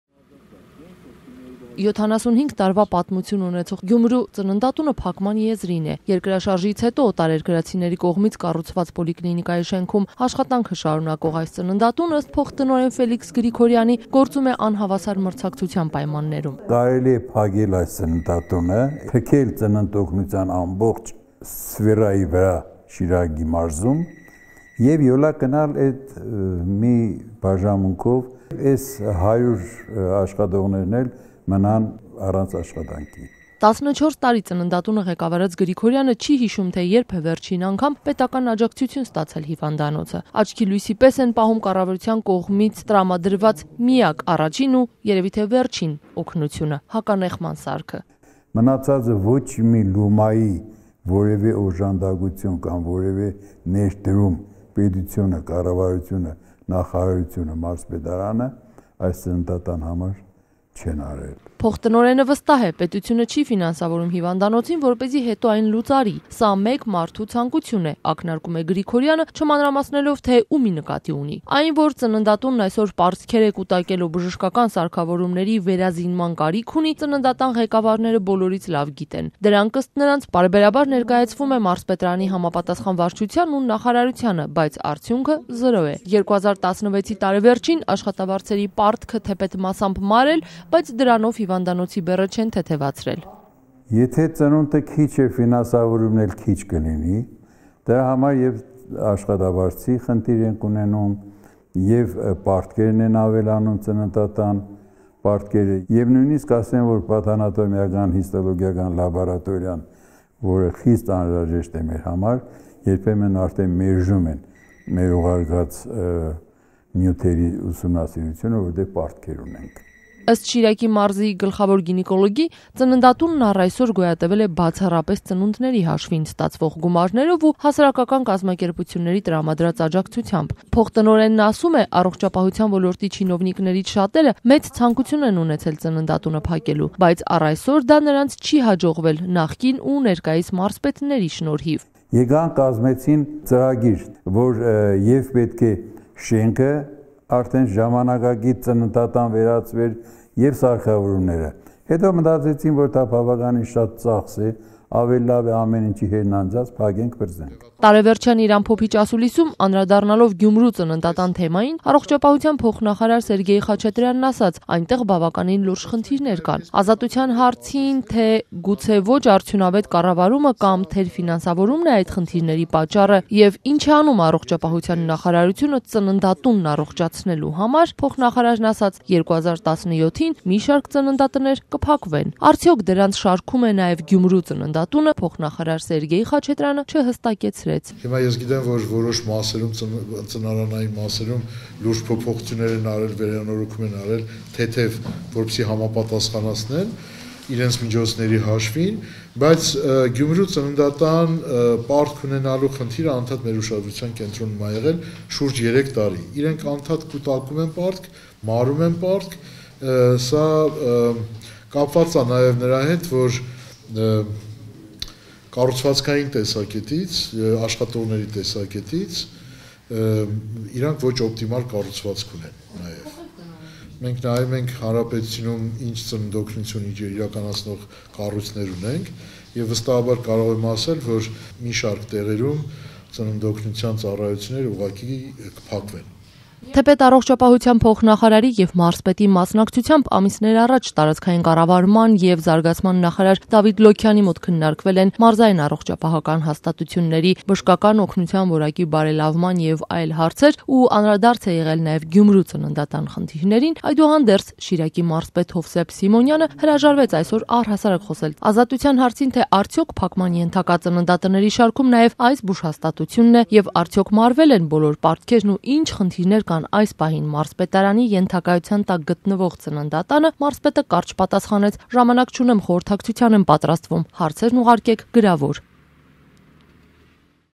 Գյումրու 75-ամյա ծննդատունը փակման եզրին է
Ծննդատան փոխտնօրենի խոսքով, առողջապահության ոլորտի «չինովնկներից» շատերը ծննդատունը փակելու «մեծ ցանկություն ունեն», բայց առայսօր դա չի արվել «միայն նախկին ու ներկա մարզպետների միջամտության շնորհիվ»: